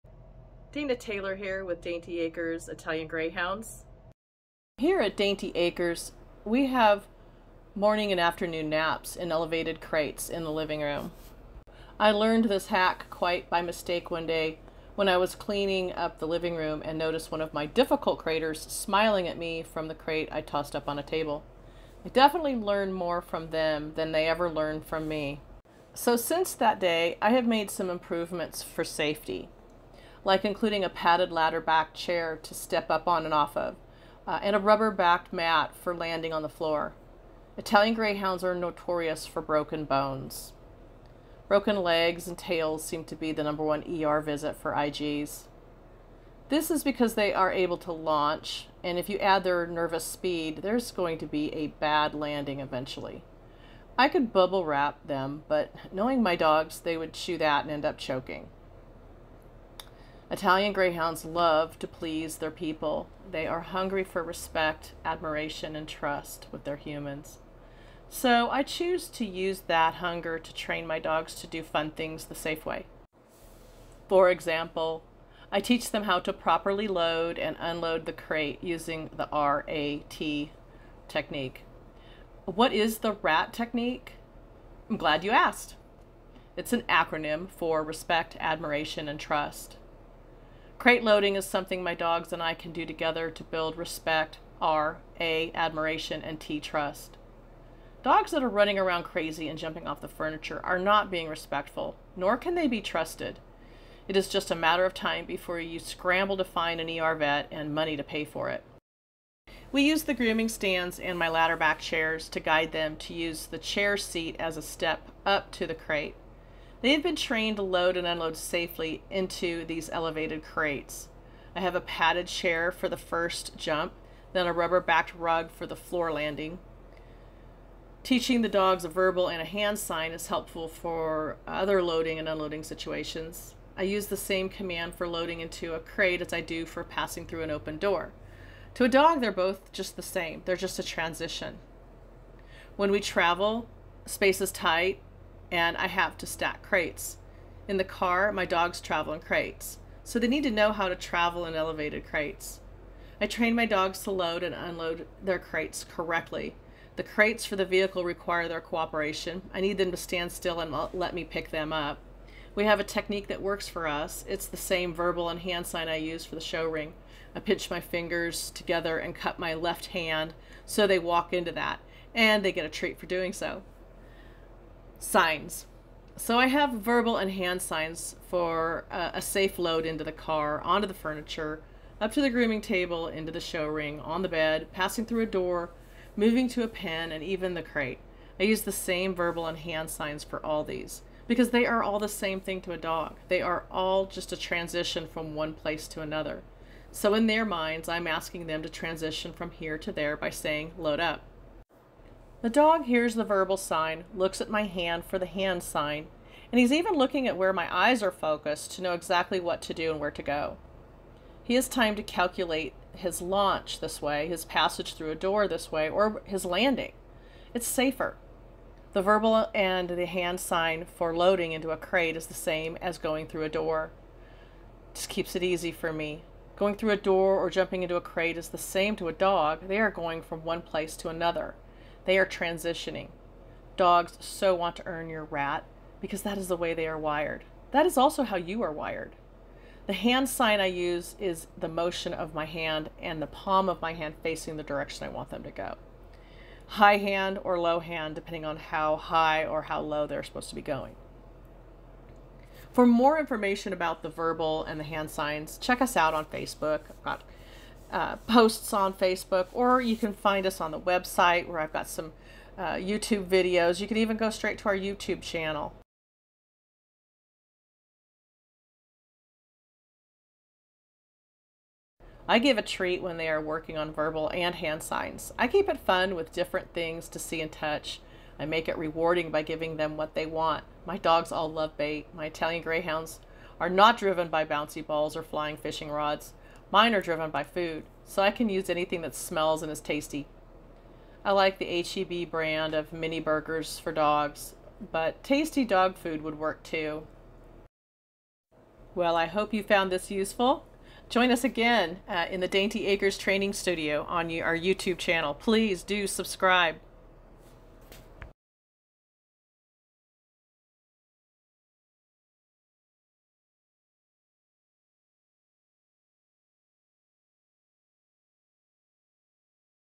Italian Greyhound Hack Series: The Elevated Crate
Dainty Acres Italian Greyhounds 12/31/22, Spring Branch Texas:
Here at Dainty Acres we have morning and afternoon naps in elevated crates in the living room. I learned this hack quite by mistake one day when I was cleaning up the living room and noticed one of my difficult craters smiling at me from the crate I tossed up on a table. I definitely learn more from them than they ever learn from me.

Safety:
So since that day, I have made some improvements for safety, like including a padded ladder-back chair to step up and off of, and a rubber backed mat for landing on the floor. Italian Greyhounds are notorious for broken bones. Broken legs and tails seem to be the #1 ER visit for IG's. This is because they are able to launch, and if you add their nervous speed; there is going to be a bad landing eventually. I could bubble wrap them, but knowing my dogs, they would chew that and end up choking. Italian Greyhounds love to please their people, they are hungry for respect, admiration and trust with their humans. So I choose to use that hunger to train my dogs to do fun things the safe way. For example, I teach them how to properly load and unload the crate using the R-A-T Technique. What is the RAT Technique? Glad you asked. It is an acronym for Respect, Admiration and Trust. Crate loading is something my dogs and I can do together to build R-respect, A-admiration and T-trust. Dogs that are running around crazy and jumping off of furniture are not being respectful, nor can they be trusted. It is just a matter of time before before you scramble to find an ER vet and money to pay for it.

 signs + fun + treats = RAT!  and Safety!

We use the grooming stands and my ladder back chairs to guide them to use the chair seat as a step up to the crate.  They have been trained to load and unload safely into these elevated crates.  I have a padded chair for the first jump, then a rubber backed rug for the floor landing.  Teaching dogs a verbal and hand sign is helpful for other loading or unloading situations.  I use the same command for loading into a crate as I do for passing through an opened door.  To a dog, they are both just a transition.

When we travel space is tight and I have to stack crates.  In the car, my dogs travel in crates, so they need to know how to travel in elevated crates.  I train my dogs to load and unload their crates correctly.  The crates for the vehicle require their cooperation.  I need them to stand still and let me pick them up.  We have a technique that works for us, it is the same verbal and hand sign I use in the show ring.  I pinch my right fingers together and cup my left hand, and they walk into that.  And get a treat for doing so!
Signs:
So I have verbal and hand signs for a safe load into the car, on the furniture,  up to the grooming table, into the show ring, on the bed, passing through a door,  moving to a pen and even the crate.  I use the same verbal and hand sign for all these, because they are all the same thing to a dog.  They are all a transition from one place to another.  So in their minds, I am asking them to transition from here to there by saying "Load-up".

The dog hears the verbal sign, looks at my hand for the hand sign and where my eyes are focused and knows exactly what to do and where to go.  He has time to calculate his launch, passage or landing.  Safer!  The verbal and hand sign for loading into a crate is the same as going through a door.  Going through a door or jumping into a crate is the same to a dog, they are going from one place to another, they are "transitioning".  Dogs soooo want to earn your RAT, because that is the way they are wired.  That is how you are wired too.  The hand sign I use is the motion of my hand and the palm of my hand facing the direction I want them to go.  High hand or low hand, depending on high or low they are going.  For more information about verbal and hand signs check us out on FB or our website at DaintyAcres.com or on Youtube

Fun & Treats:
I give a treat when we are working on verbal and hand-signs. I keep it fun with different things to see and touch, and I make it rewarding by giving them what they want. My dogs all love bait. My Italian Greyhounds are not driven by bouncy balls or flying fishing rods. Mine are driven by food! So I can use anything that smells and is tasty. I like the HEB brand of Mini Burgers for dogs. But tasty dog food would work too.

Well I hope you found this helpful!  Join us again in the Dainty Acres Training Studio!  @ItalianGreyhounds